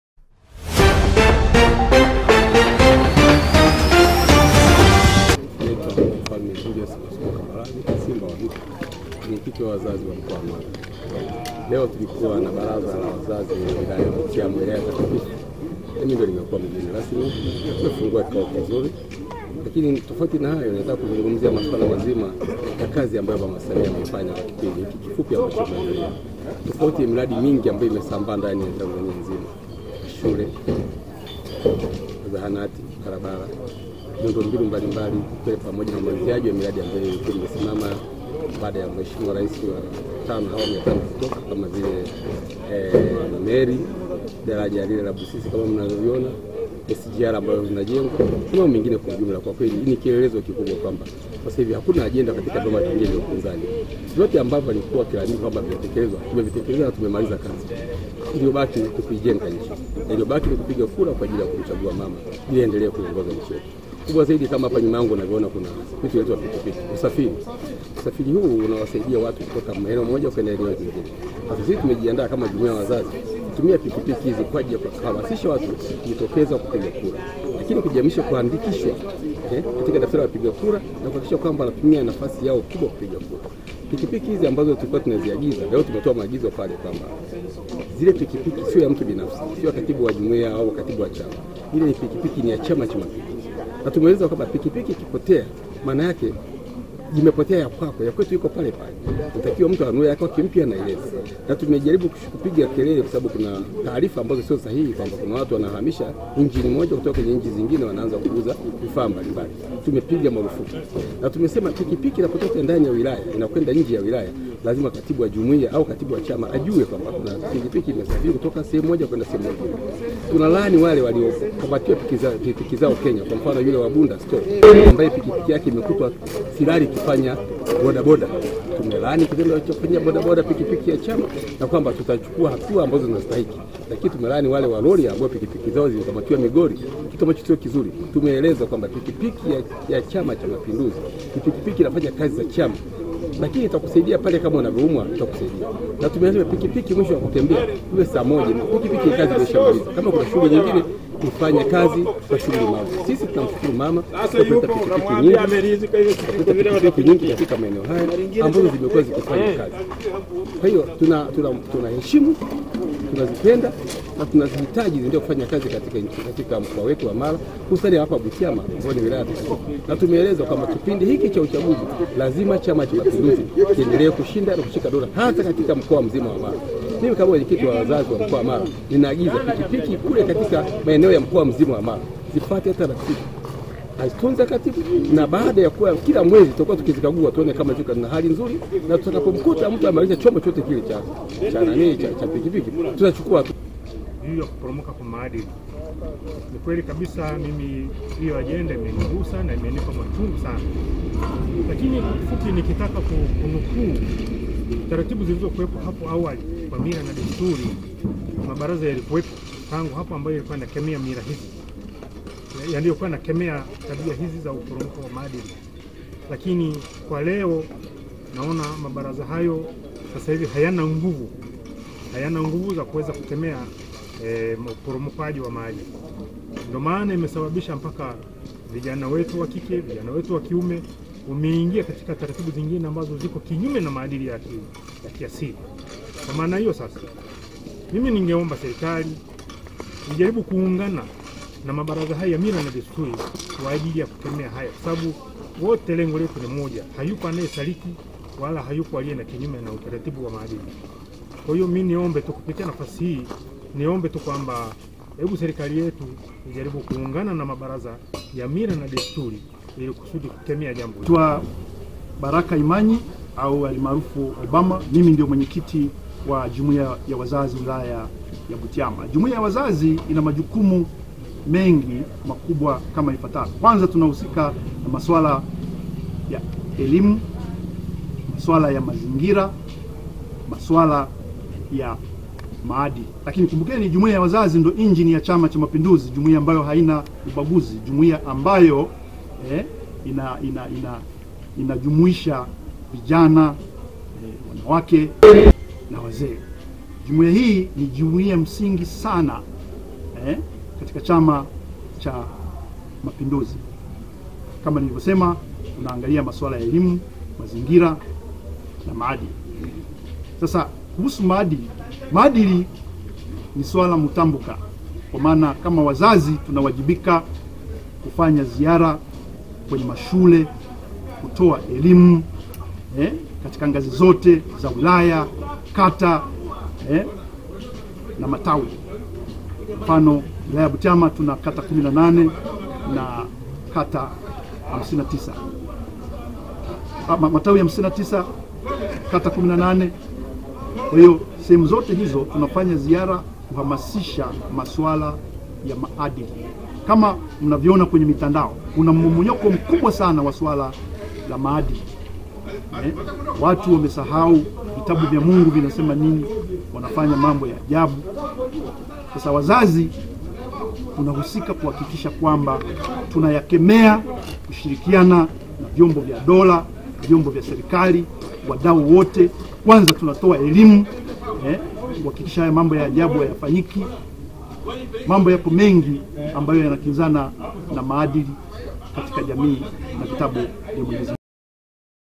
JUMUIYA YA WAZAZI MARA YASISITIZA MATUMIZI SAHIHI YA PIKIPIKI ZA CHAMA.
BUTIAMA MARA